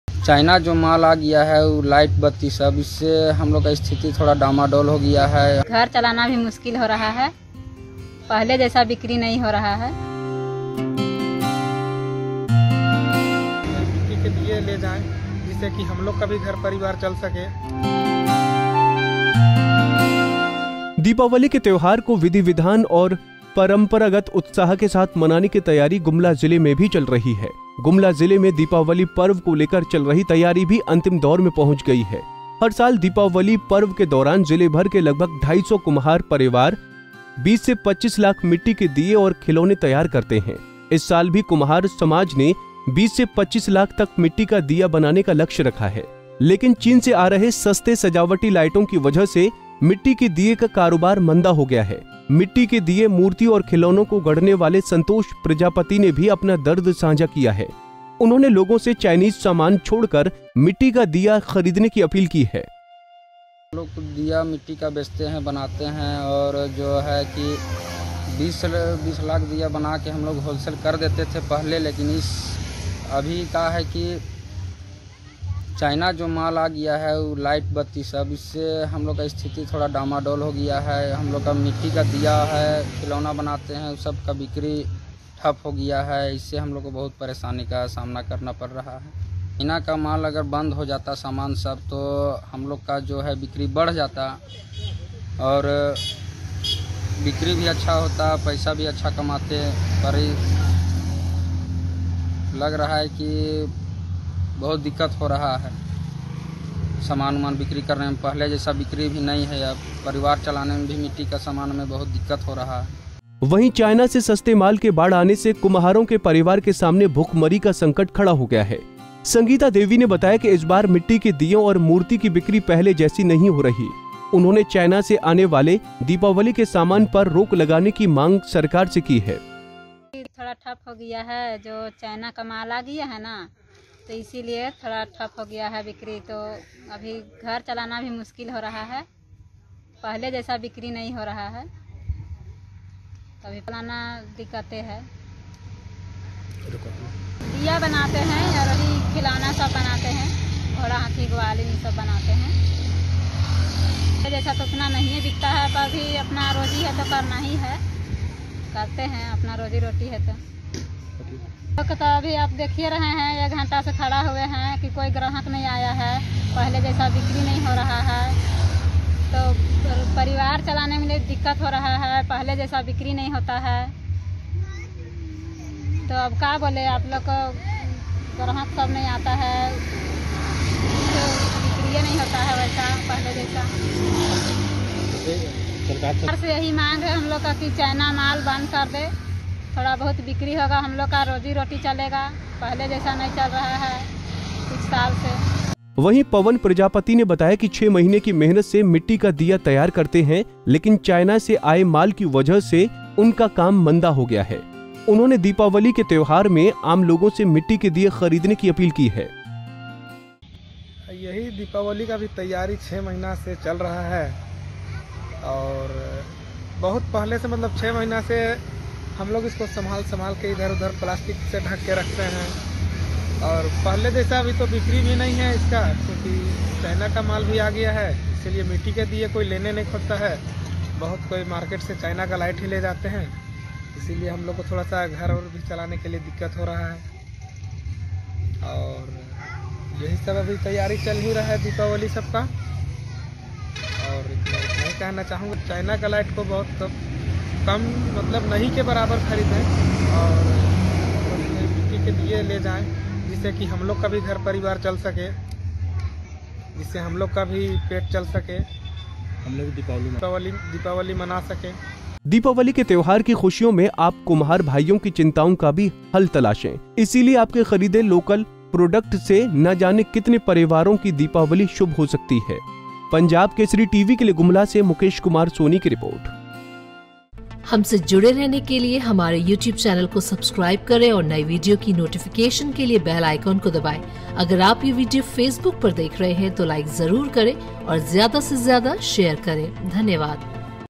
दीपावली में कुम्हारों के बनाए दिए और मूर्तियों की करें जमकर खरीदारी
दीपावली में कुम्हारों के बनाए दिए और मूर्तियों की करें जमकर खरीदारी, चाइना के सस्ते सामान को छोड़ बनें लोकल फोर वोकल कैंपेन का हिस्सा